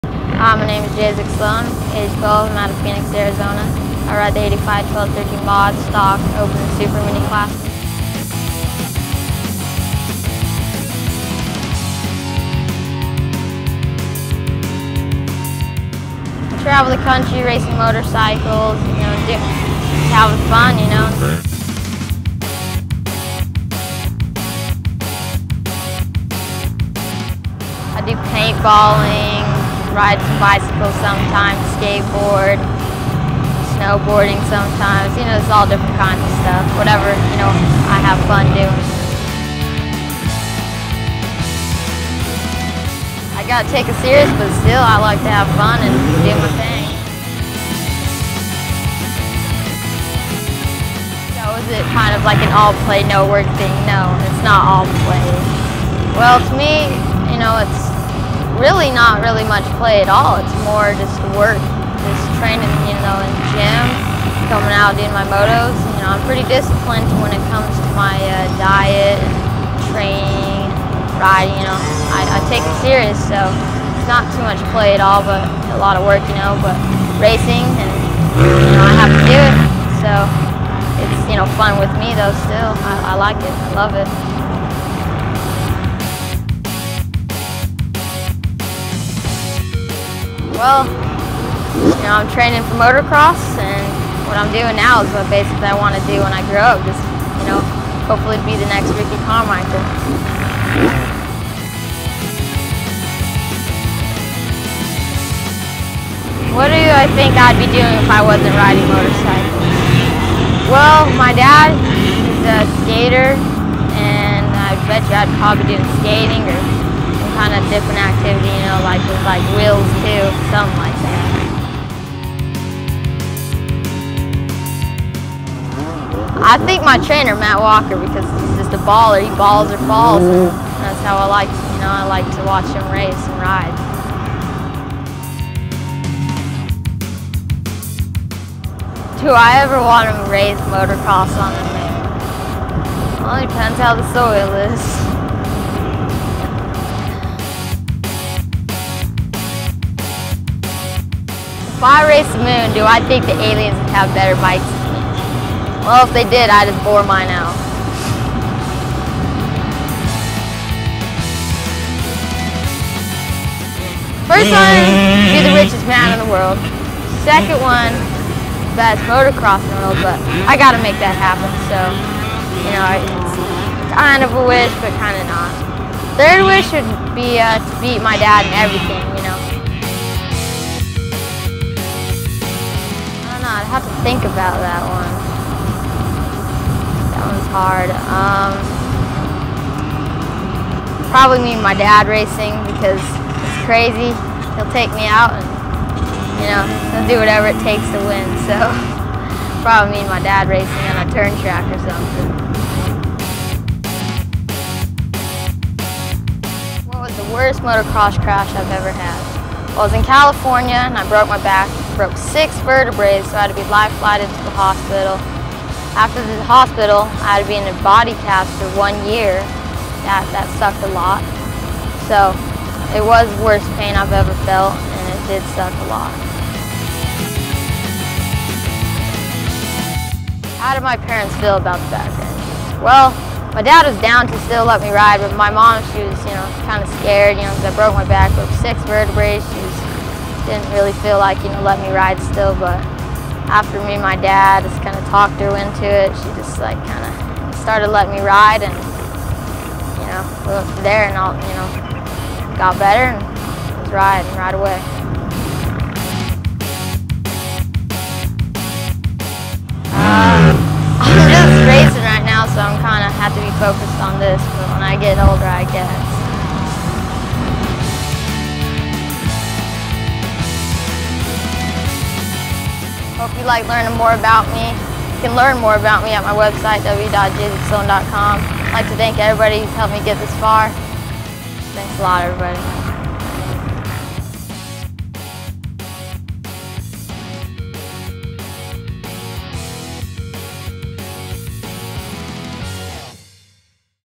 Jaisaac Sloan Motocross Interview
Jaisaac Sloan A motocross interview On himself, Family, girls, and Aliens.  Holla if you feel em!!!